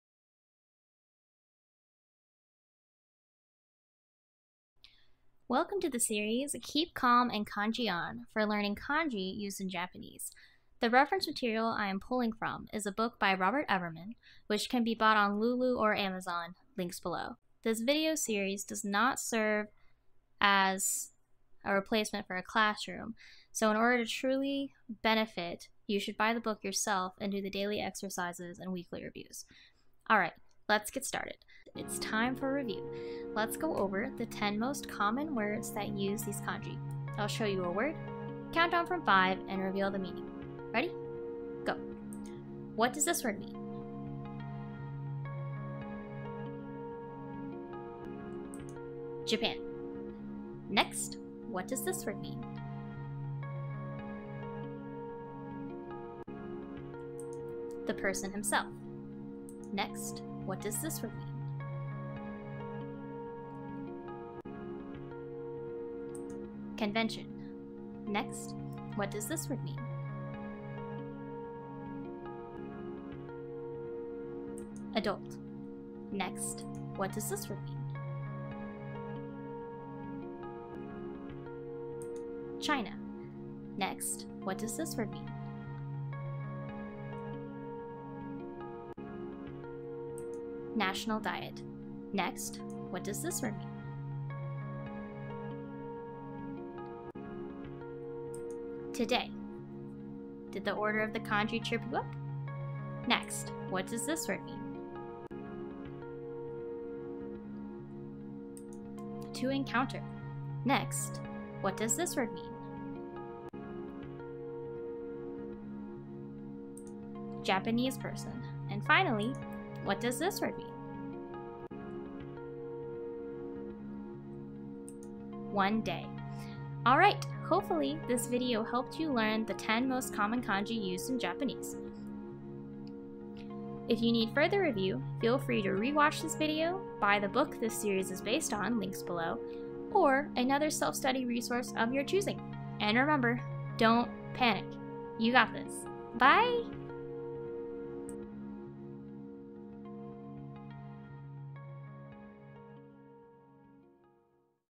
CinnaMonBon Learns Kanji [Review]: Characters 1-10 (日人一大出本年国中会)
Hi everyone!

Today's episode is a review on the top 10 most common kanji used in Japanese (日人一大出本年国中会).

This video series is based on the book "Keep Calm and Kanji" by Robert Everman.